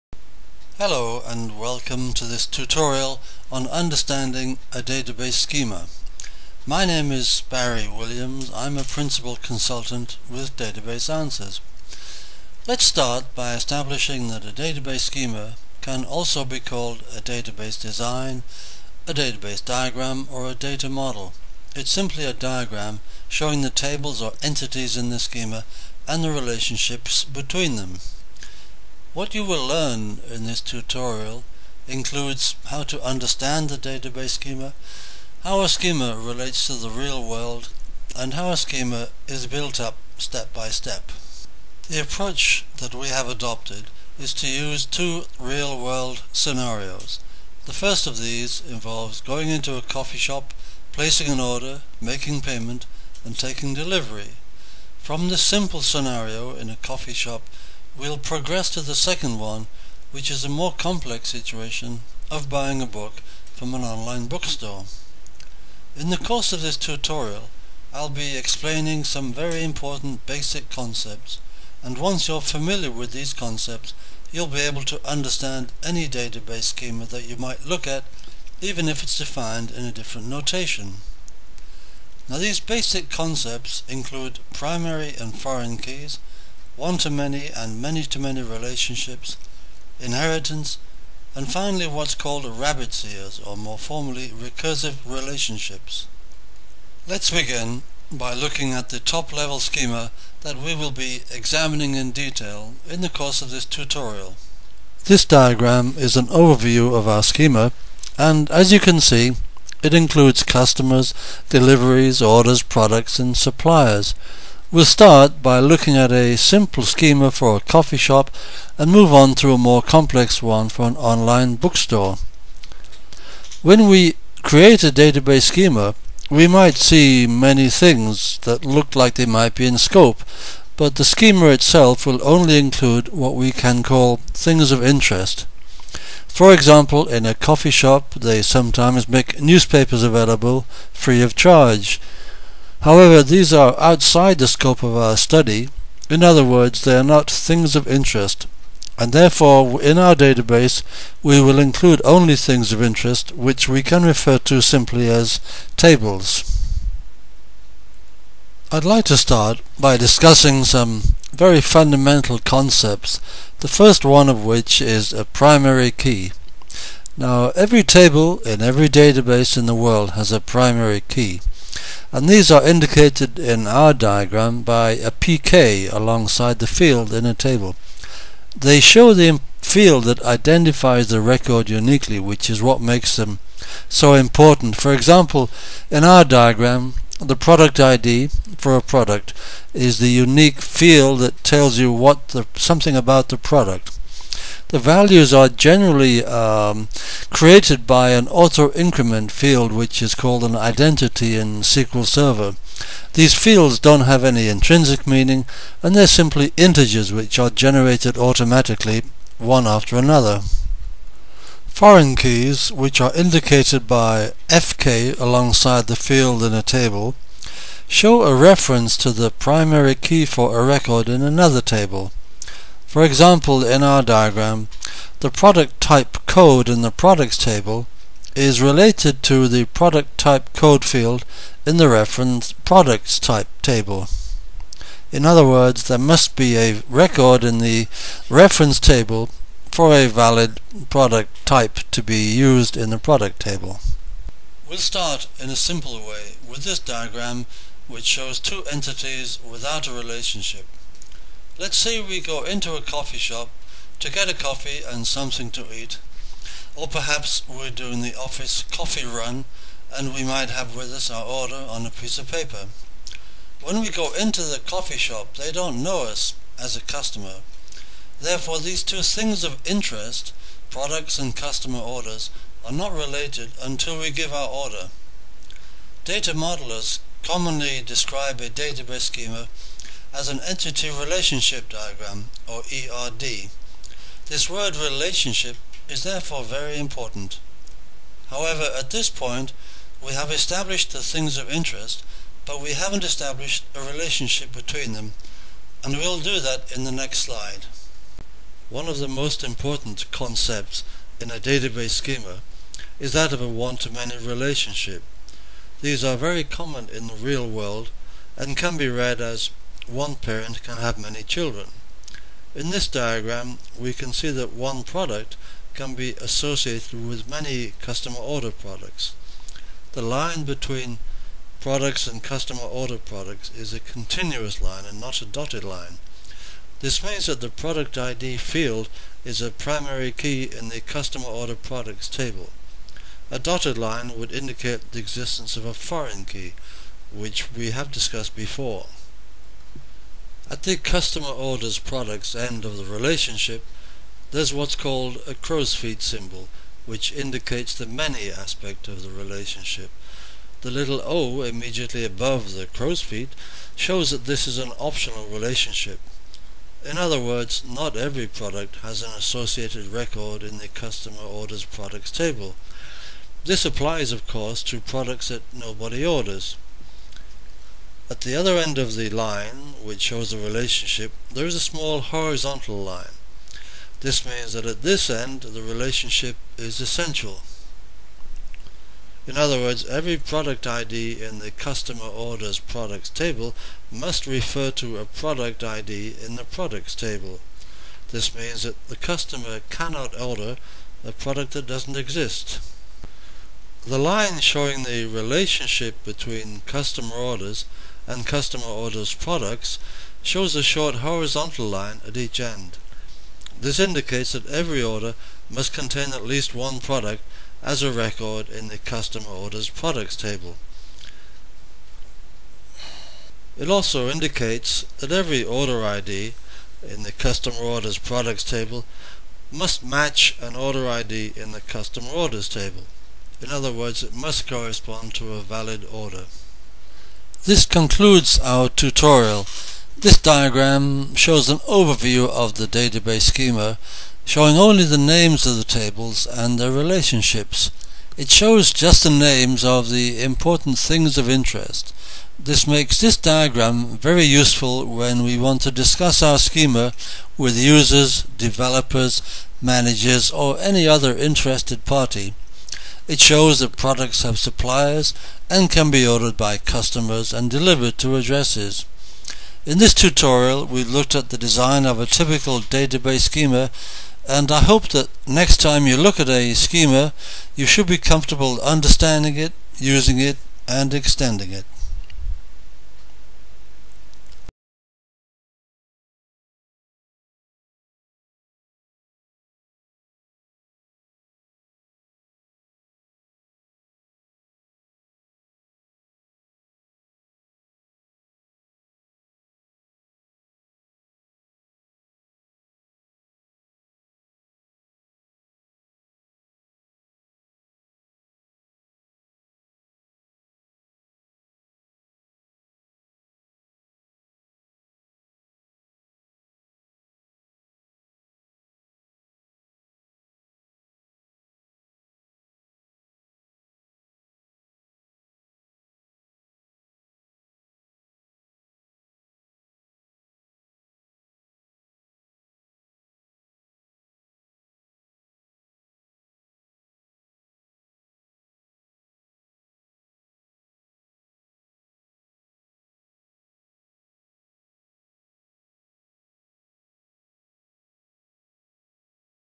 Barry's Tutorial on understanding a Database Schema
A 15 minute version of this Tutorial is available on the Microsoft Web Site at this page :-

and is also listed on my Web Site.